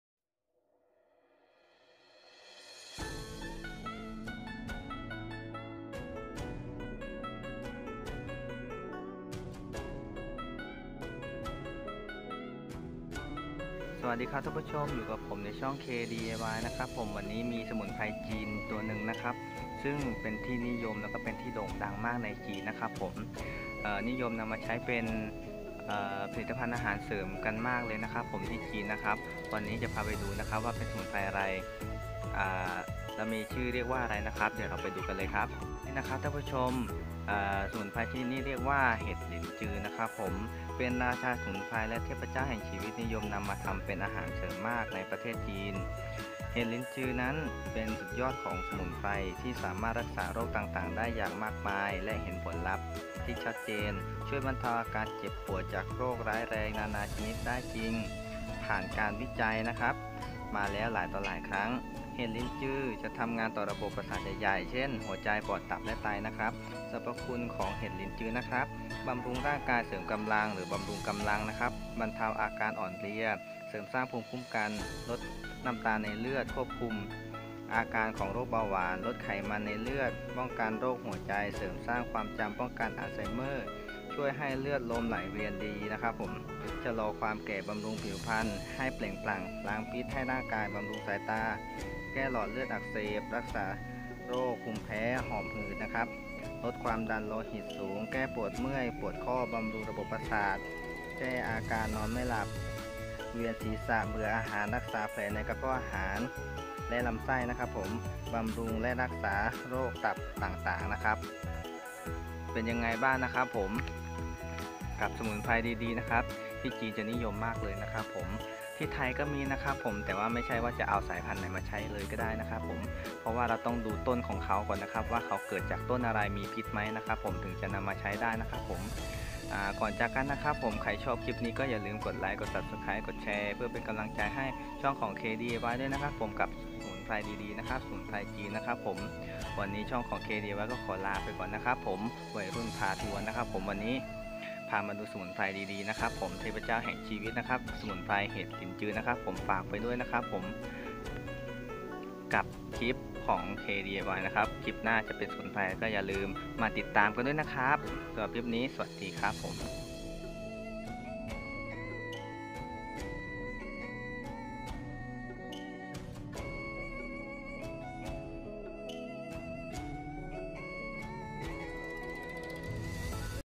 สมุนไพรเห็ดหลินจือ ราชาสมุนไพรจีนเทพเจ้าแห่งชีวิต
สมุนไพรจีนที่มาแรงในตอนนี้มีสรรพคุณมากมายและมีคุณค่าสูง